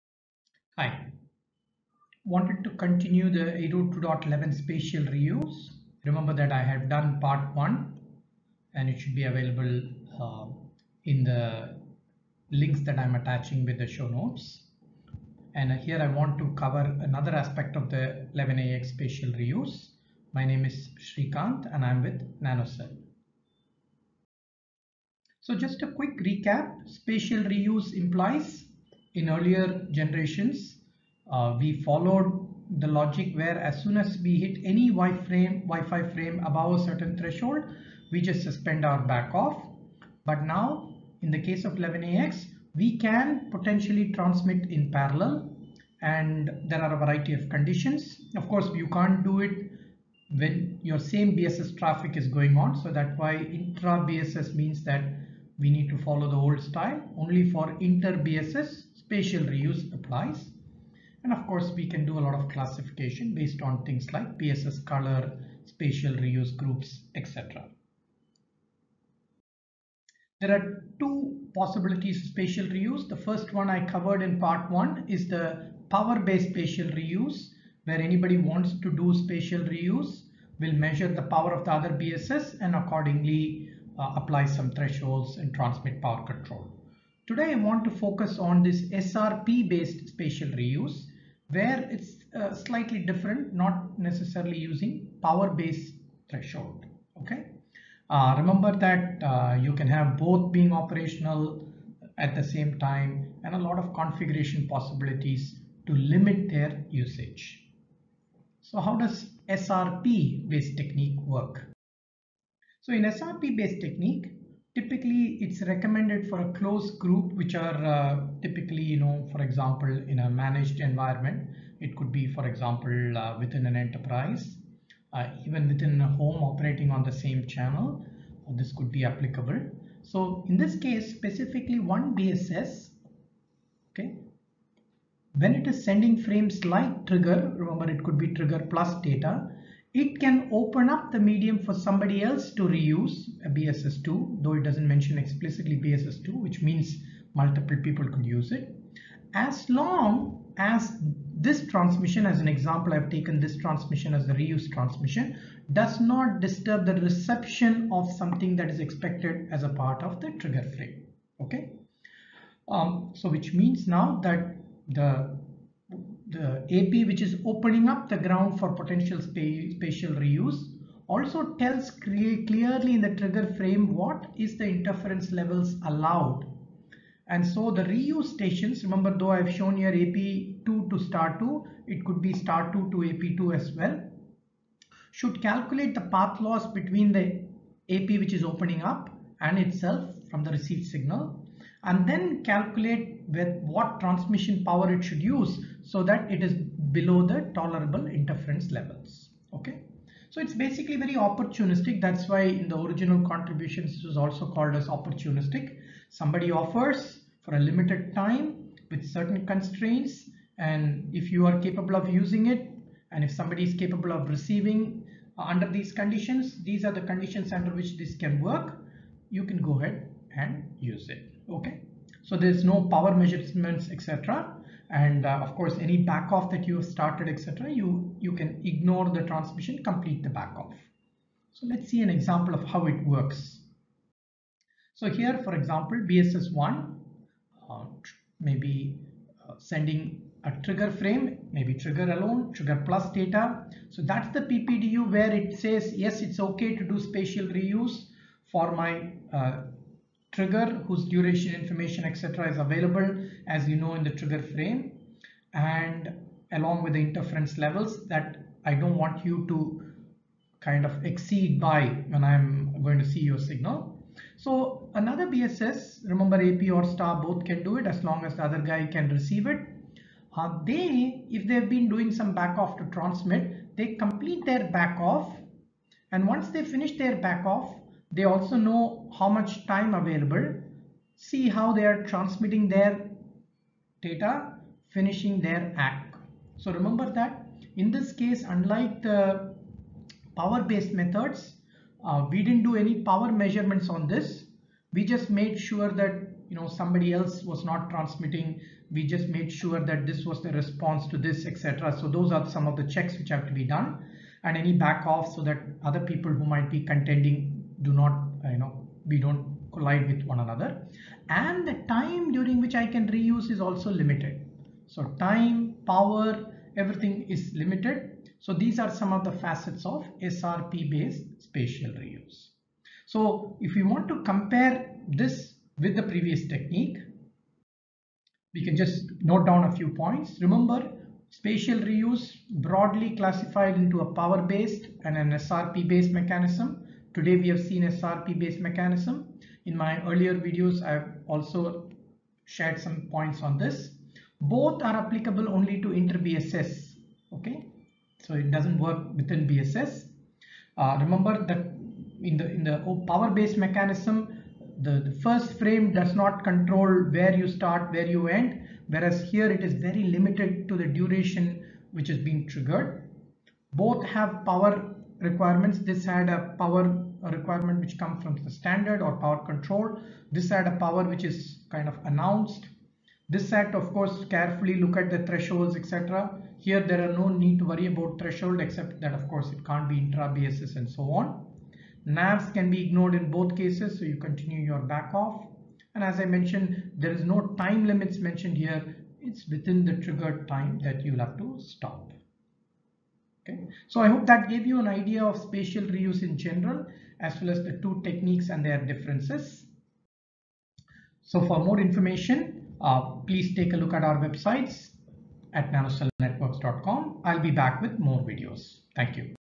802.11ax Spatial Reuse - Part 2
In Part 2 of the spatial reuse features, we continue from where we left off in Part 1 which is covered here
We cover the SRP based spatial reuse based on opportunistic access to the channel.  We also cover differences between the power based and SRP based methods.